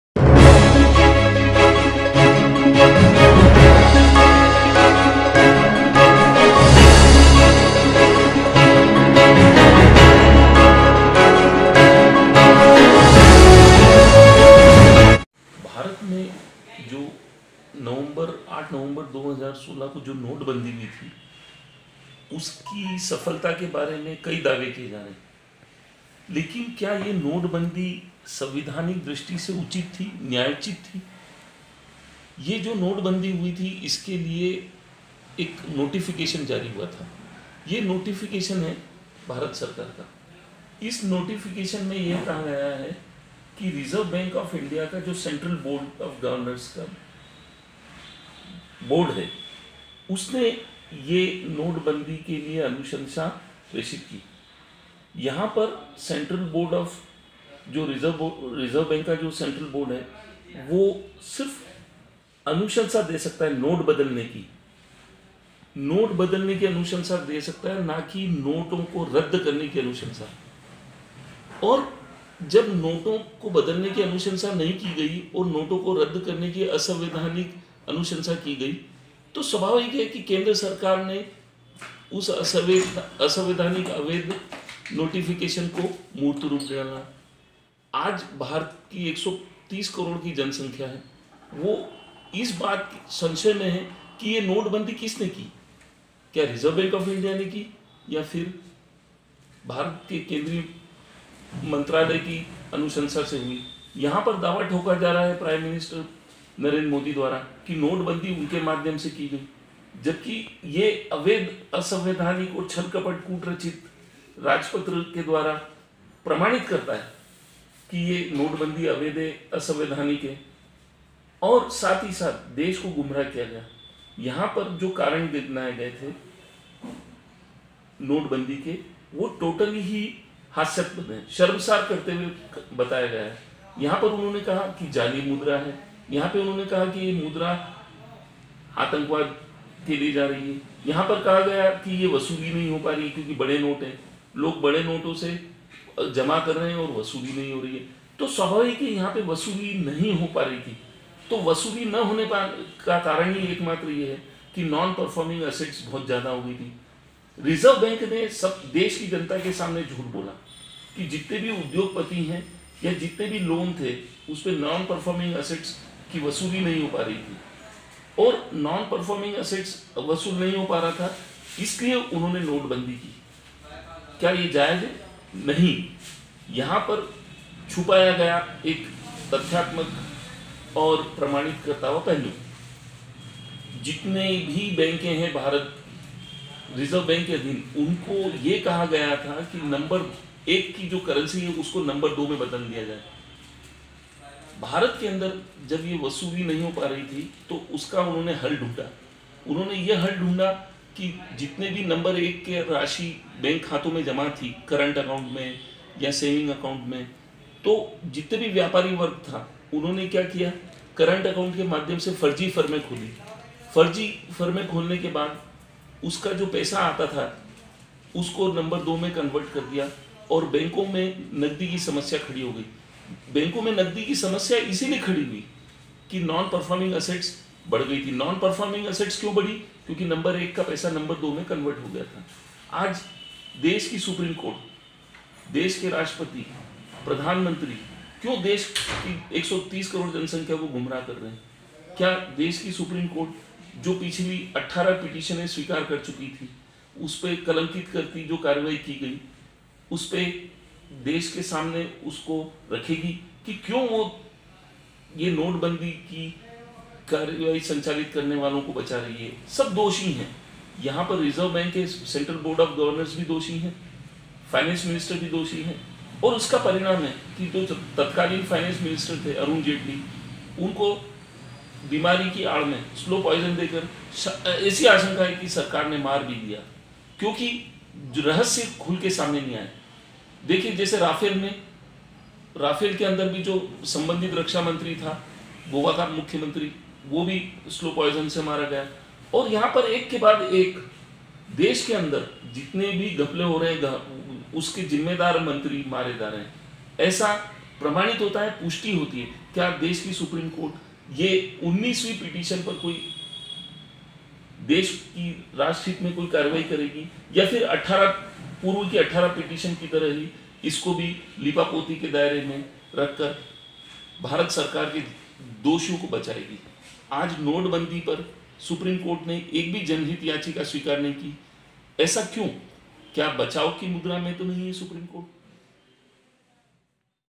नोटबंदी में सुप्रीम कोर्ट, दोषियों को बचाने की मुद्रा में आखिर क्यो??
क्या सुप्रीम कोर्ट 19वी याचिका स्वीकार करेगा - प्रभात भट्ट??